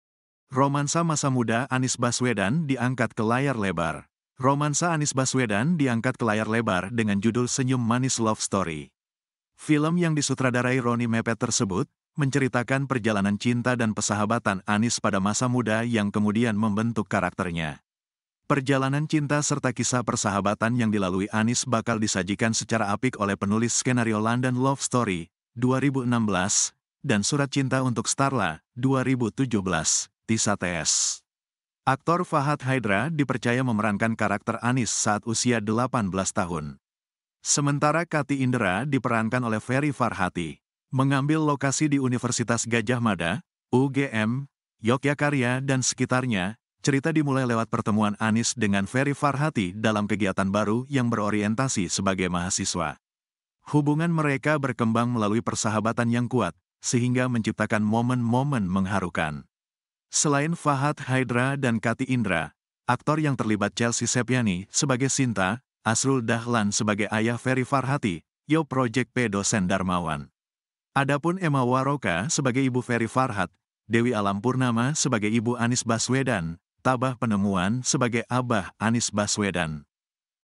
Romansa Masa Muda Anies Baswedan Diangkat ke Layar Lebar
Romansa Anies Baswedan diangkat ke layar lebar dengan judul Senyum Manies Love Story. Film yang disutradarai Ronny Mepet tersebut, menceritakan perjalanan cinta dan pesahabatan Anies pada masa muda yang kemudian membentuk karakternya.